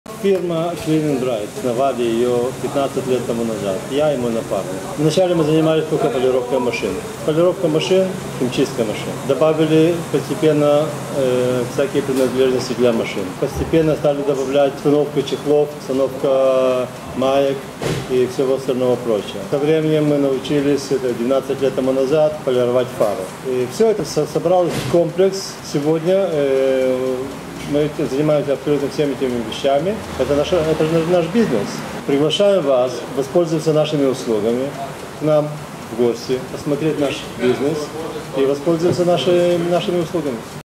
автосервис по полировки машин вступительное слово хозяина
(мы находимся в хайфе,нешер,раён тель ханан,в нутри тех осмотра "тест кар") тел:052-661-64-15 и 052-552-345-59
в этом видео владелец автосервиса по полировки машин расскажет покажет в мельчайших подробностях о полировки на автомобилях а также даст ценные эксклюзивные советы.видео будет полезно и авто любителям и профи...
пожалуйста оставьте коментарии.